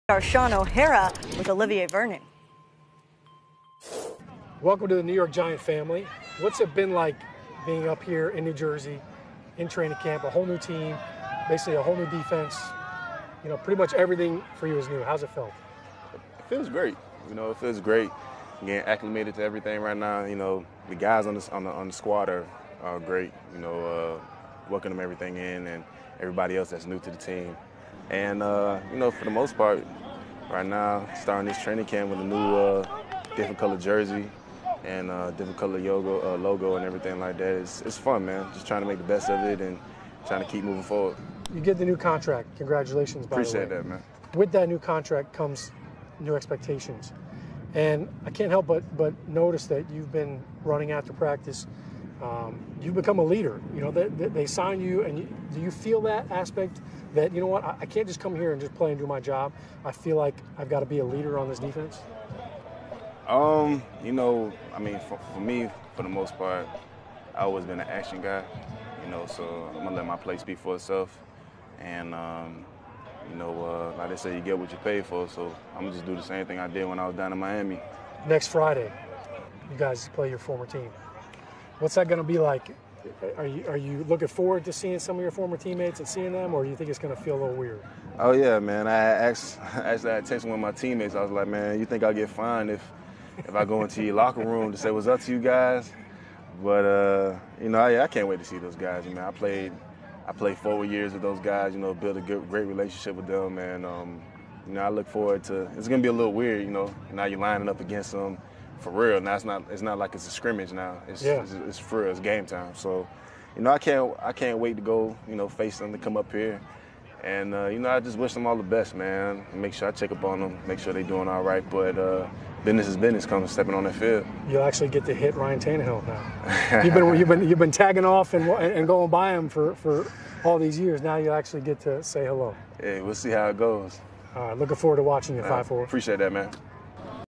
Olivier Vernon: I'm going to let my play speak for itself
Shaun O'Hara talks one on one with defensive end Olivier Vernon about his new role with the New York Giants.